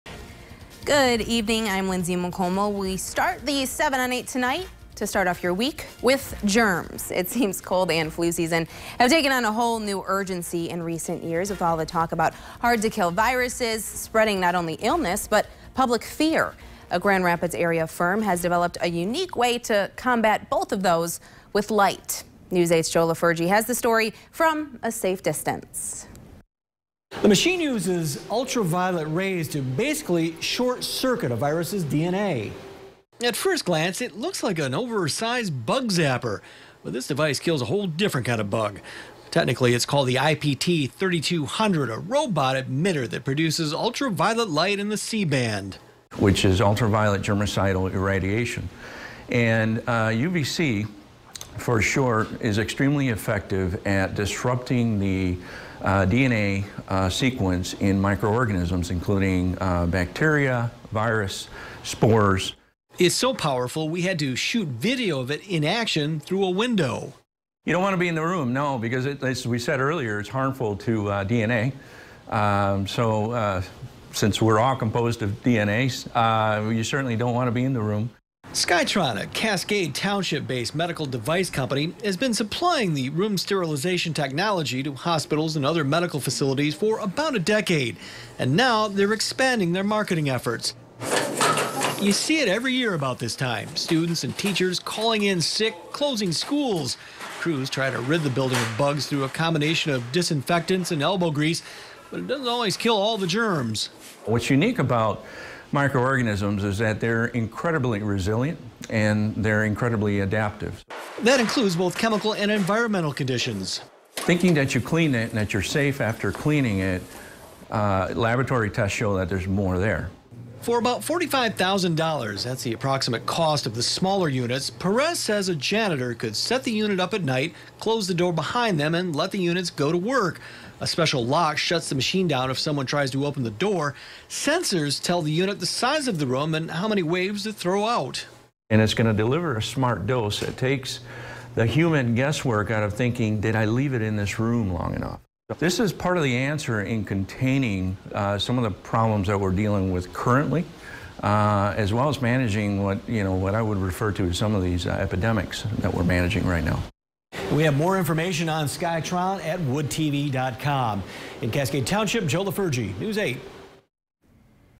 Local company using UV rays to fight the flu
Amid talk about hard-to-kill viruses spreading not only illness but also public fear, a metro Grand Rapids firm has turned to an unexpected way to combat the problem: light. (Jan. 27, 2020)